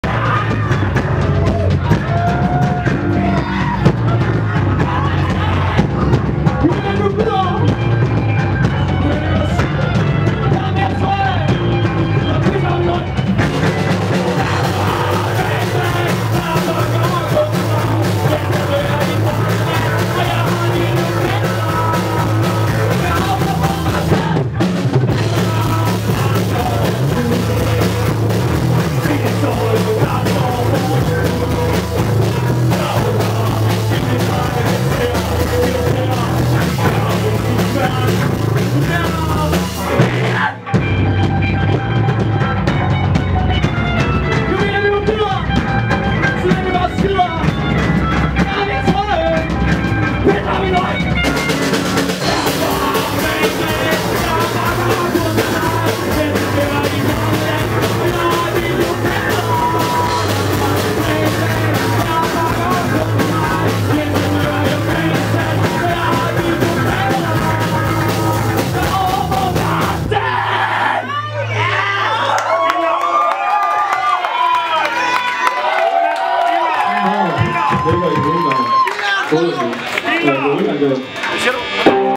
De Sorte Huller @ Musikcafeen - 23/03/2011
Danish Punk Band named "De Sorte Huller".
A very aggressive, original and manic debut-performance, though it had a ironic and tragicomic touch to it.
De Sorte Huller have their roots in the Aarhus underground scene, but seems to be making a serious step up the ladder with yesterdays performance @ Musikcafeen, Aarhus. (25/03-2011)